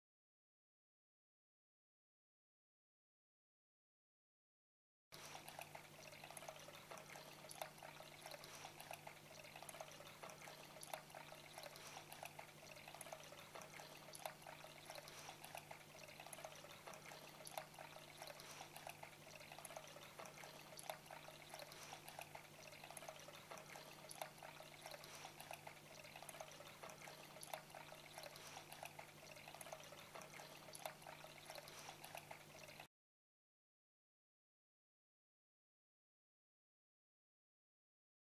High Crag Pots Water Feature
Water pours down through the various shaped and sized broken pots into the large bowl at the bottom which houses the pump and all the water.

This feature is self contained and does not need an external water supply. Simply remove from the box and plug in- within 5 minutes you will have a relaxing addition to your garden or home.

During the winter time we would recommend covering the feature with a protection cover and adding Algae Stopper to the water during the Spring/Summer months to reduce the chance of alage and bacteria build up.